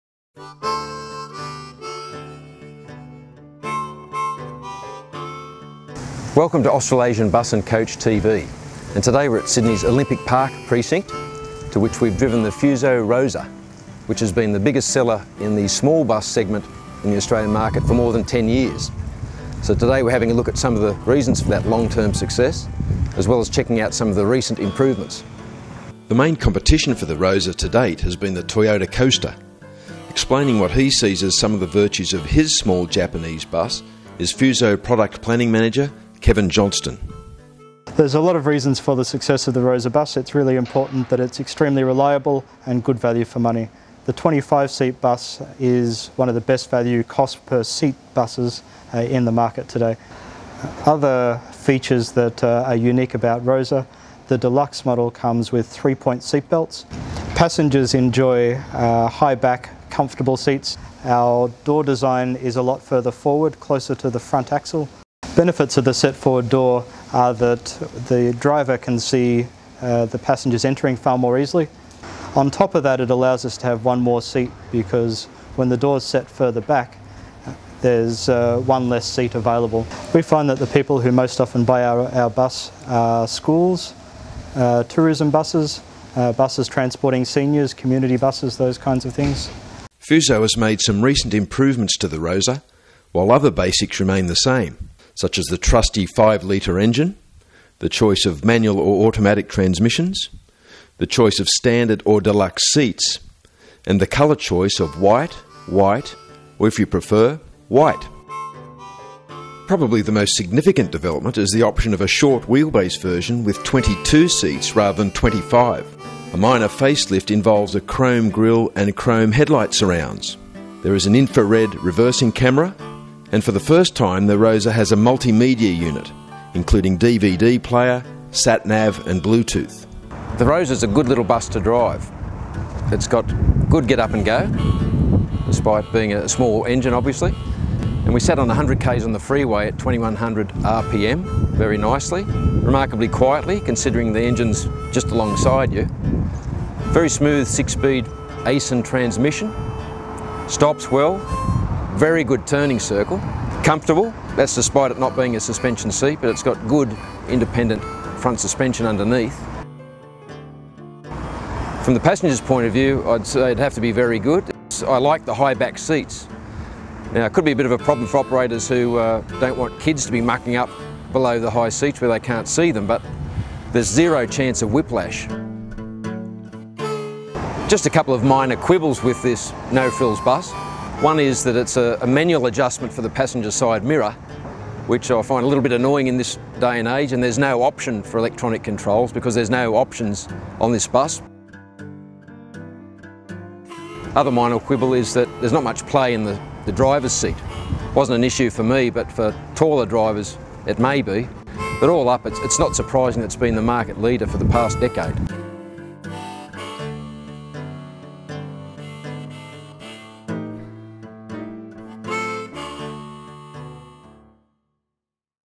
Fuso Rosa Minibus Review
The Fuso Rosa has been the market leader in Australia’s small bus segment for more than a decade.

We check out some of the Rosa’s long-term secrets to success as well as looking at some recent improvements which include a multi-media unit, reversing camera and the option of a shorter wheelbase version with 22 seats.

The latest videos from ABC are always on our main site first, have a look